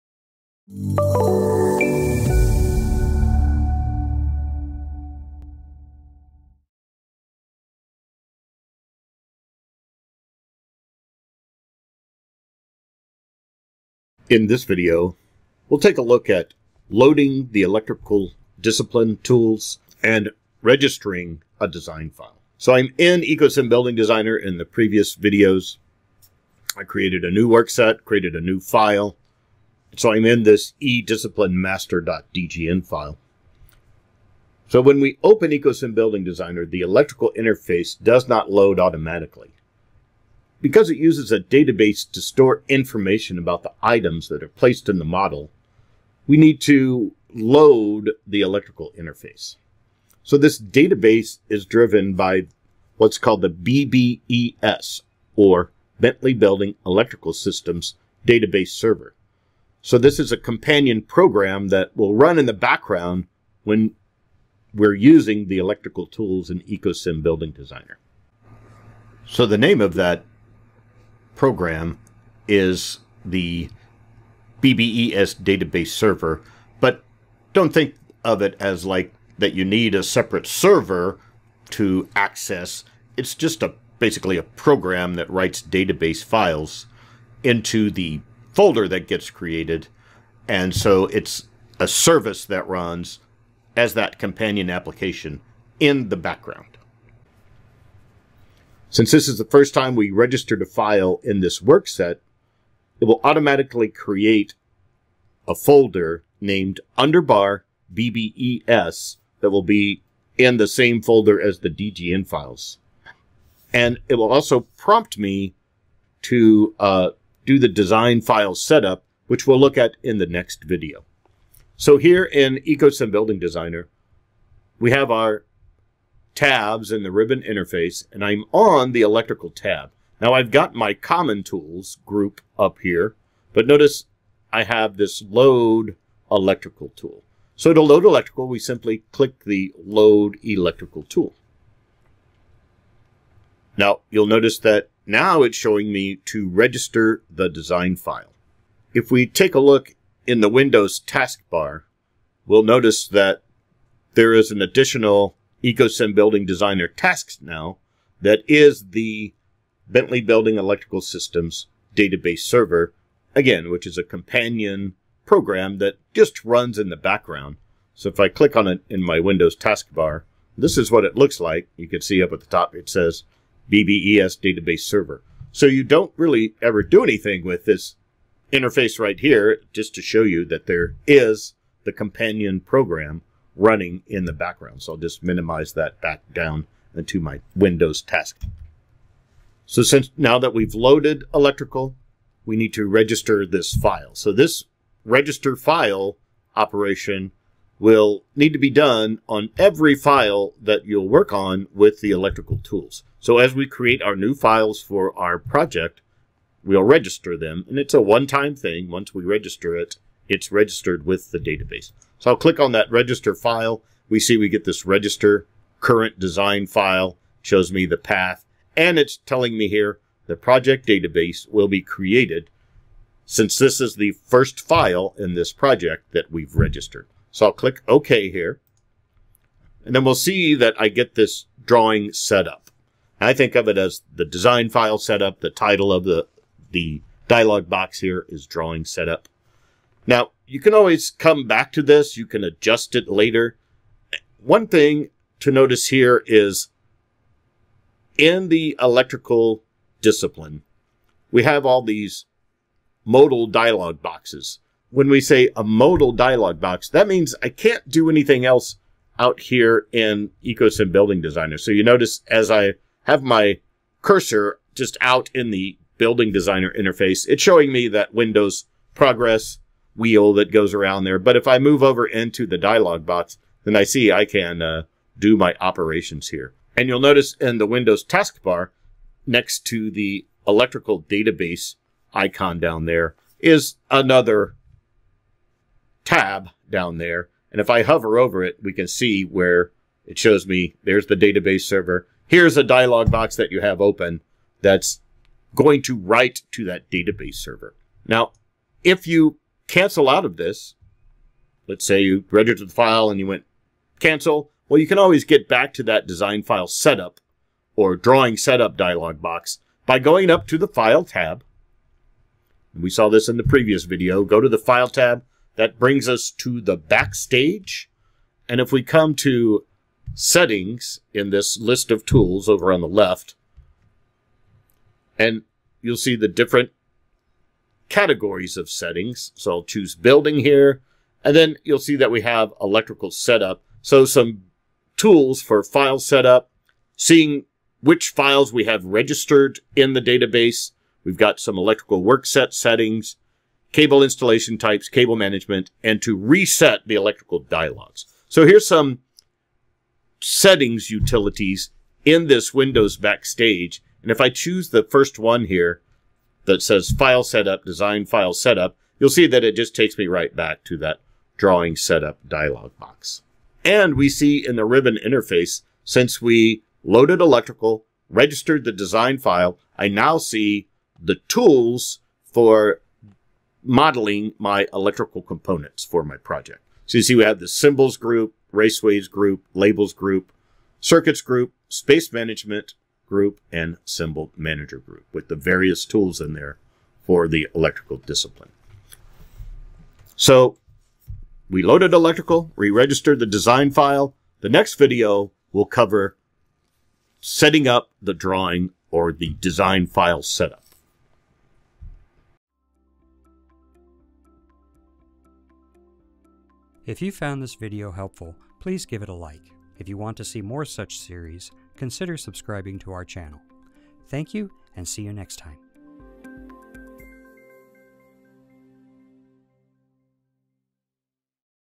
Part 3 - Loading the Electrical Tools and Registering the Design File in OpenBuildings Designer
In this video we  will cover loading the electrical discipline tools, registering electrical design files and introduce the BBES electrical database folder. To view the complete course and download a workbook visit the Bentley Learn Server.